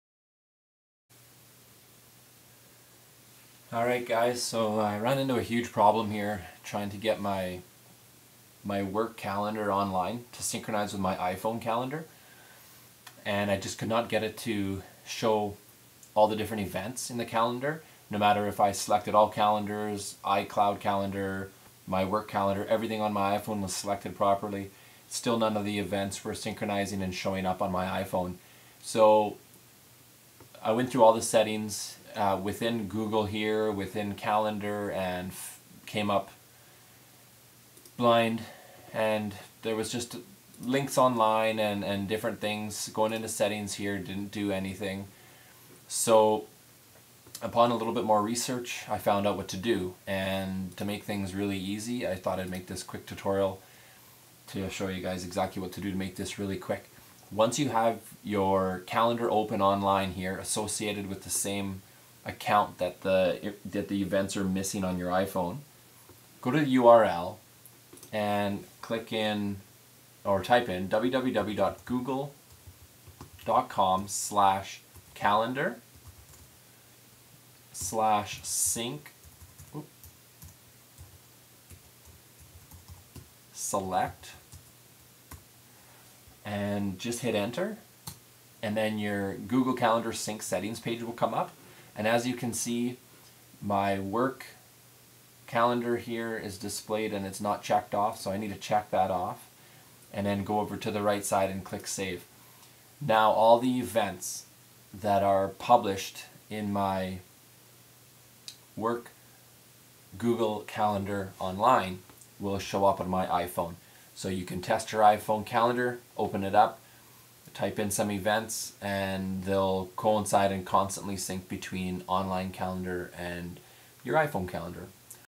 Sync Google Calendar to Iphone/ipad calendar
If you cannot get your events online to show up on your iPhone or iPad calendar even if your account is all set up check out this video to help you with a little trick.